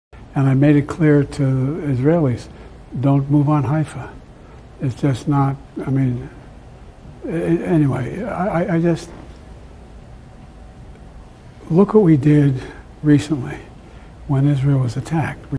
President Biden's Warning To Israel
President Biden told Israel not to attack Haifa, the third largest city in Israel.